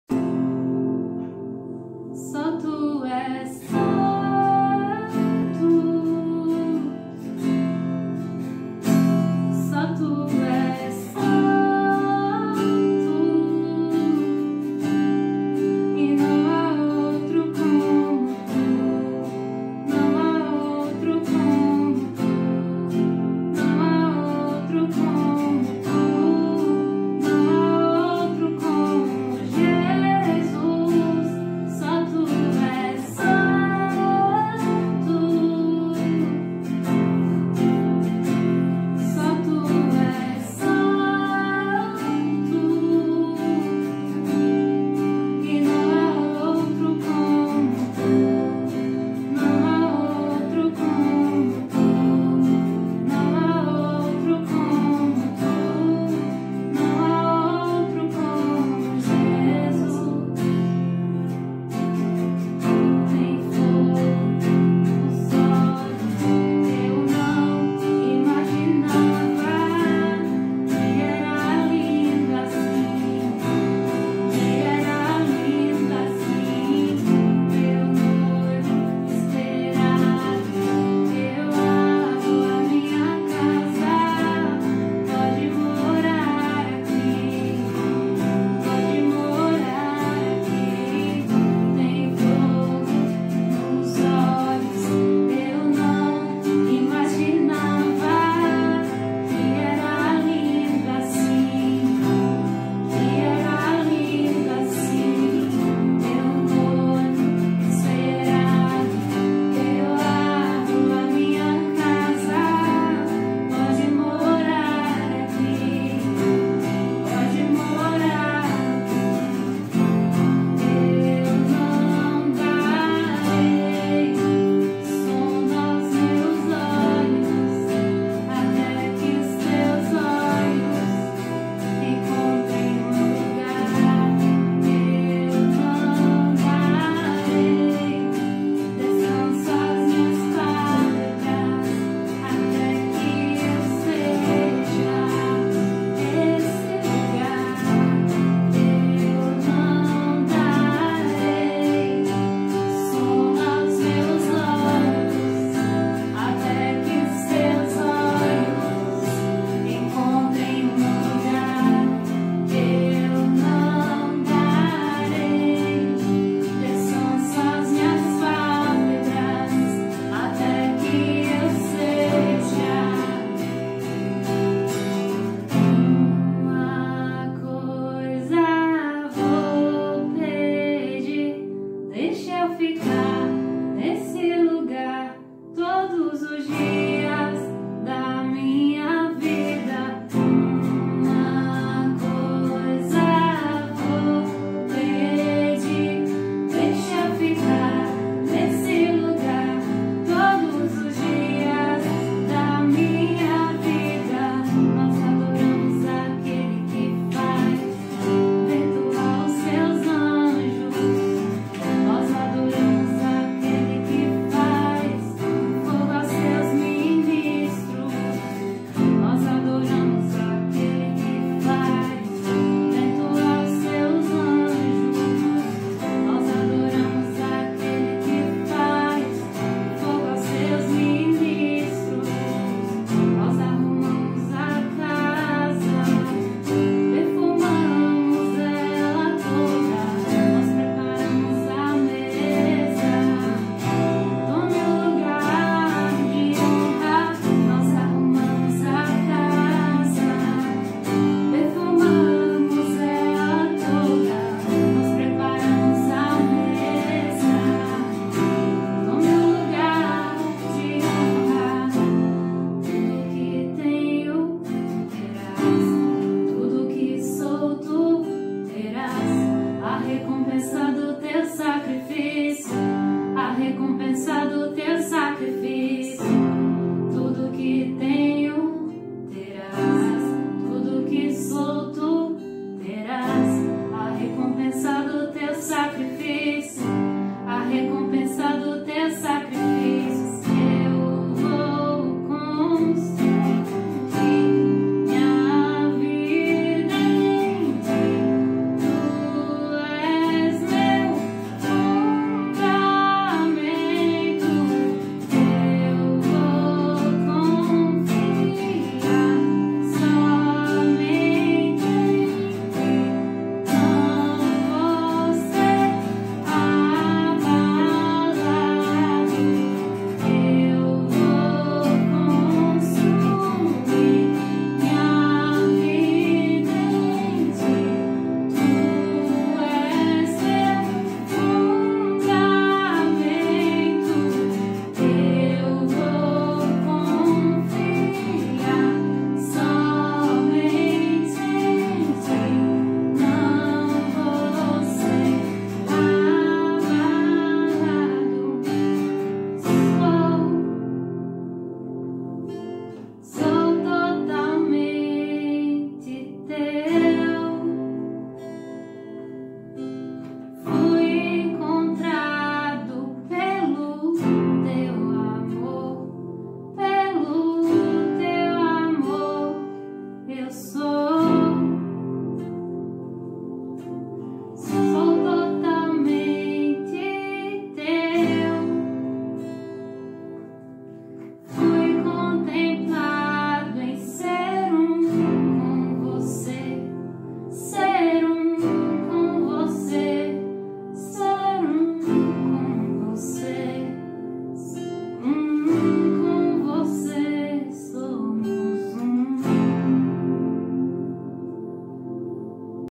Medley Morada, Theo Rubia, Juliano Son - Cover
Samylla Vasconcelos e Danusa Petra num medley com trechos de músicas de Morada, Theo Rubia e Juliano Son.